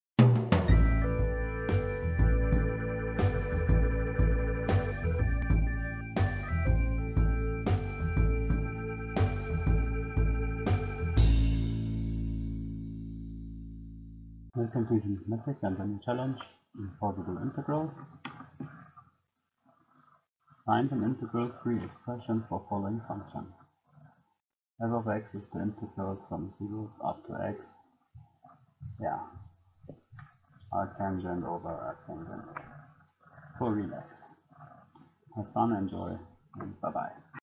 29. Challenge: Impossible integral?
Hello,

in this challenge we see a monster integral.

NOTE:
During the construction a stupid mistake happened. :( I feel so sorry about that.

Here is a correct version and its solution. :)